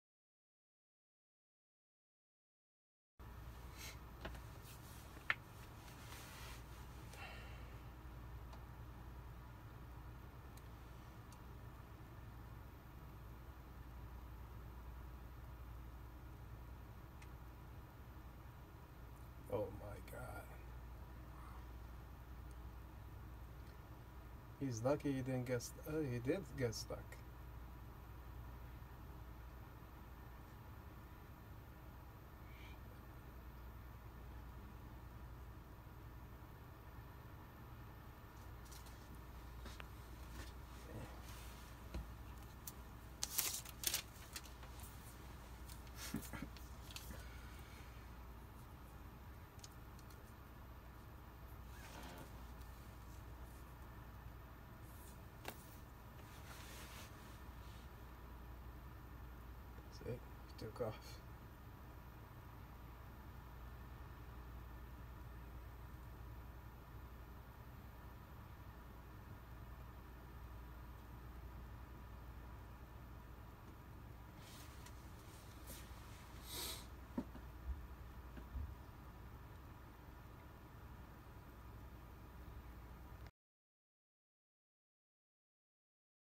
Brockton gas pump goes up in flames after driver backs into it
A driver backed into a gas pump at a Brockton gas station just before midnight on March 7.